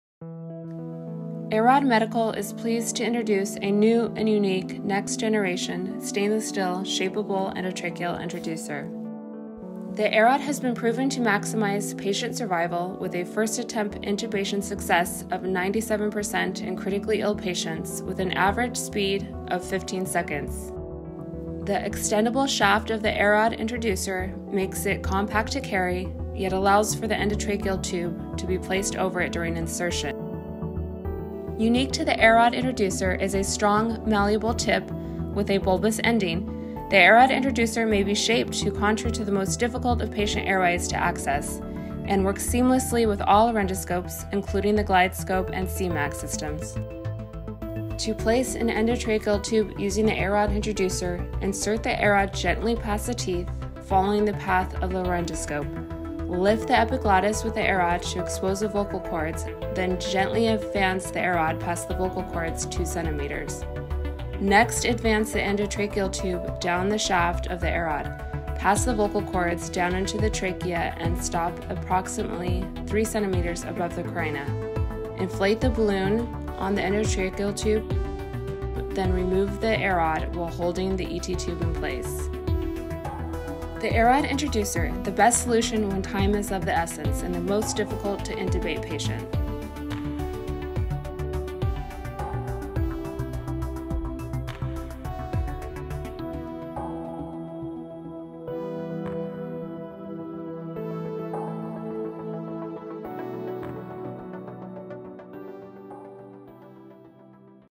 AIROD® difficult airway intubation
The AIROD® can lift the epiglottis improving the exposure of the vocal cords during endotracheal intubation.